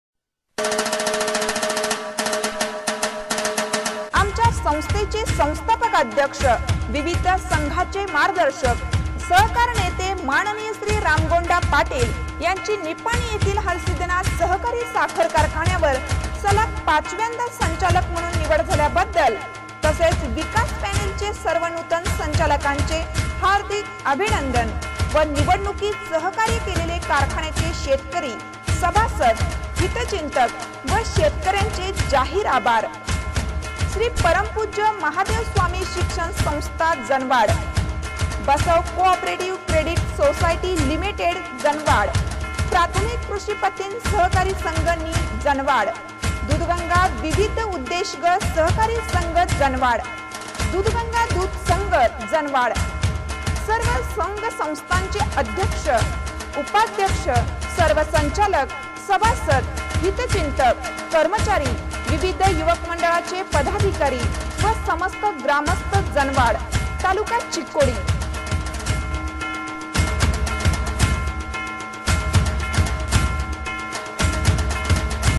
मा.श्री. रामगोंडा पाटील यांची निपाणी येथील हालसिध्दनाथ सहकारी साखर
कारखान्यावर सलग पाचव्यांदा संचालक म्हणून निवड झाल्याबददल